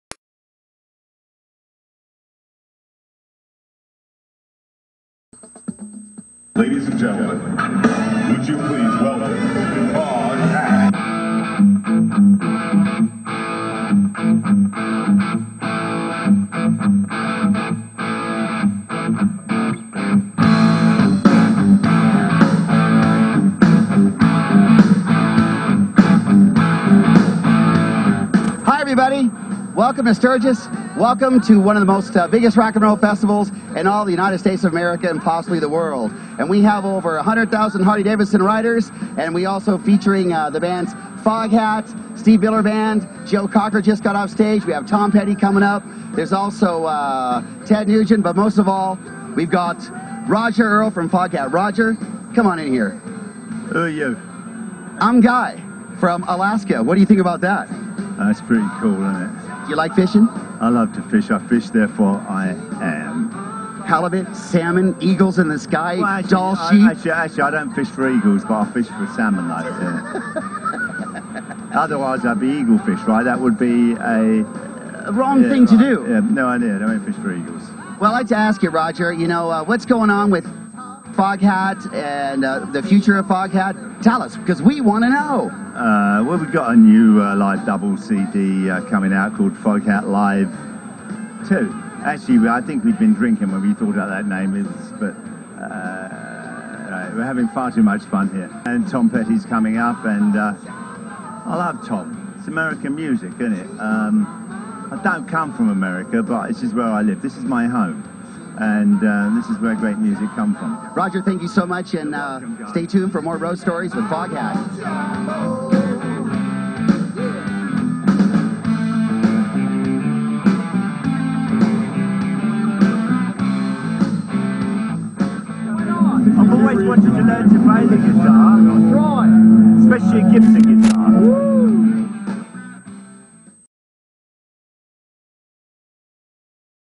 Foghat Reality Show Live Sturgis part 1 by Guy Olsen
Meet Roger Earl of  Foghat backstage in Sturgis Glencoe Campground event.  Part of a series produced by Guy Olsen called Road Stories featuring Foghat and an upclose look at its band members like never seen before.
Get to know Foghat, watch all of the series or parts.  I am producing my pilot series for my own cable or TV show.
Wish me luck and thank you Foghat for believing in me. Be sure to watch all 5 segments of Foghat Live Reality Show Sturgis.